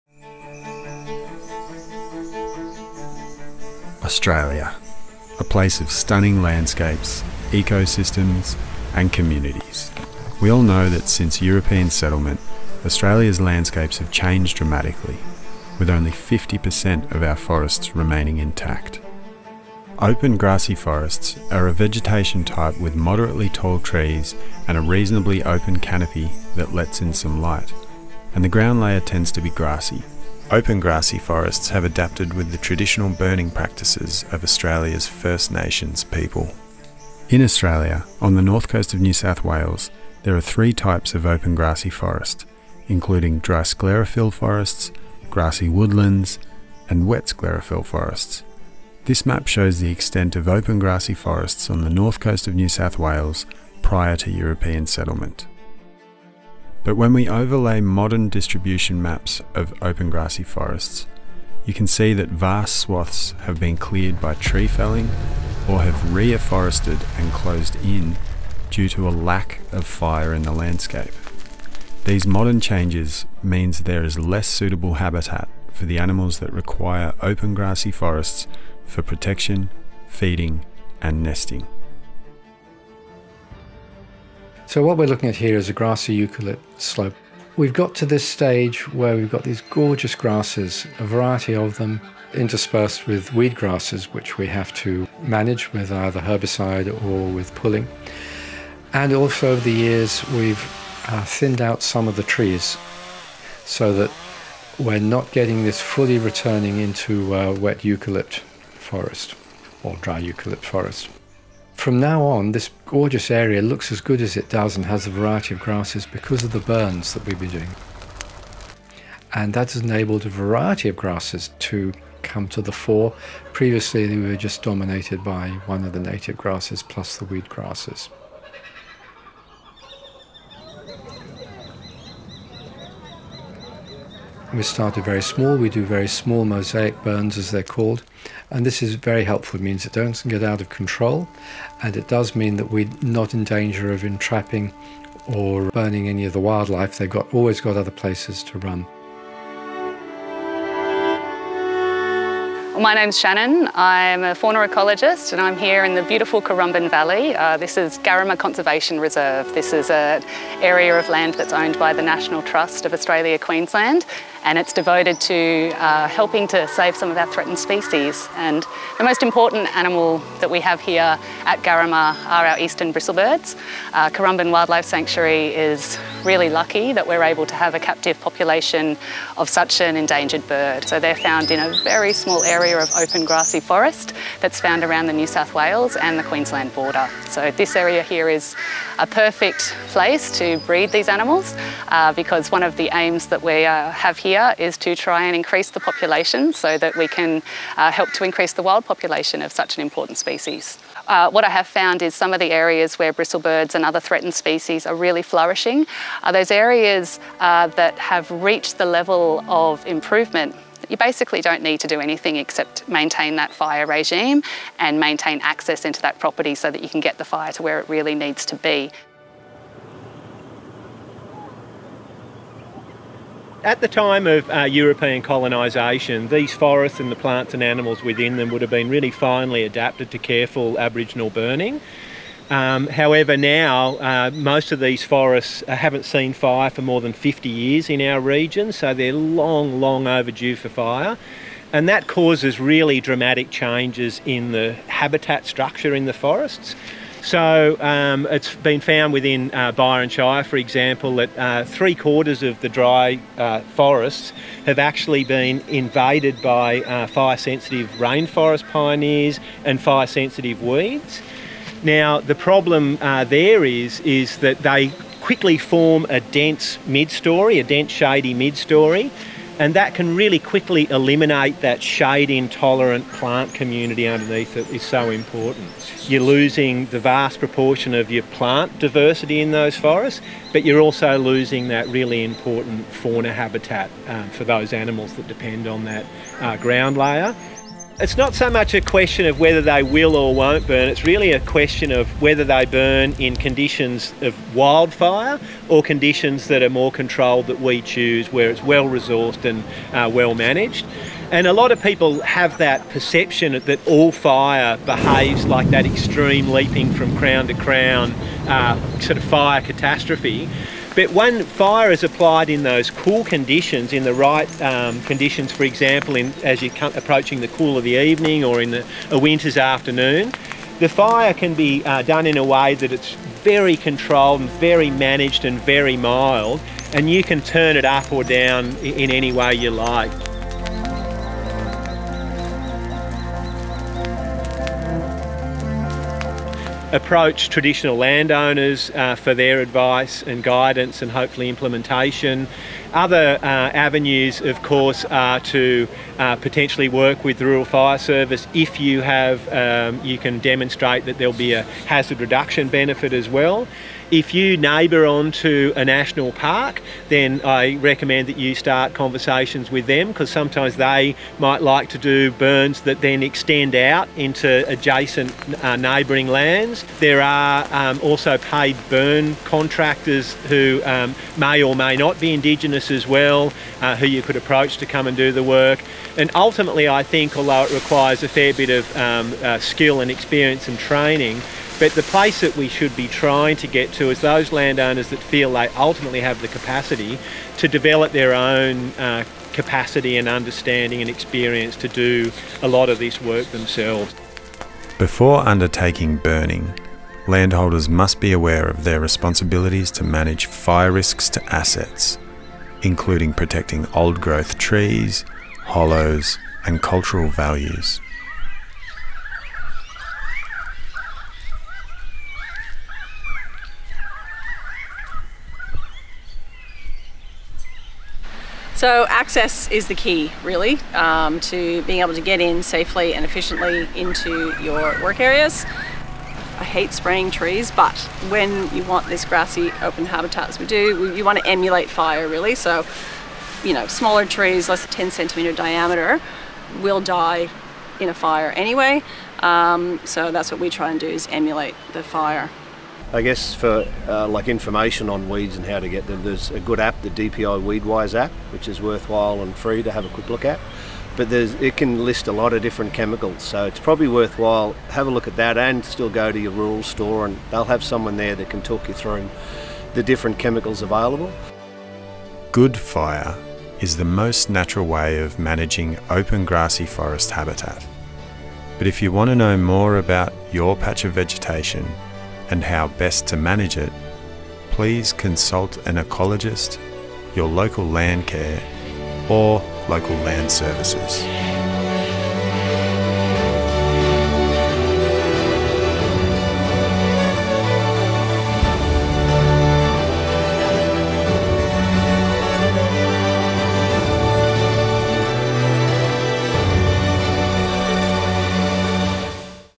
Importance of Open Grassy Forests
The North Coast Local Land Services team have been working on a project that informs landholders about the importance of Open Grassy Forests, the native and threatened species that utilise these habitats and the different management activities used to enhance and prevent further decline of these forest types.